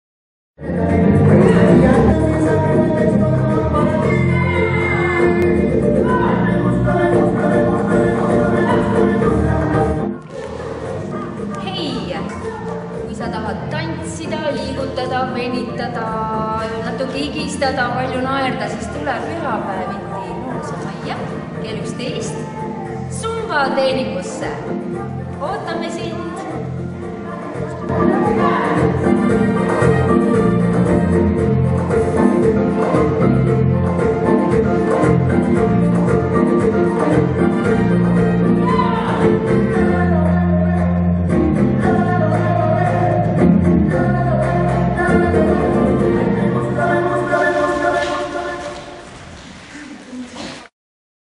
Pärnu Noorusemaja: ZUMBA Treeningud
Nooruse Maja suures saalis Zumba trenn

PÜHAPÄEVITI KL 11.00

Pääse 3 €

Treener Marge Rull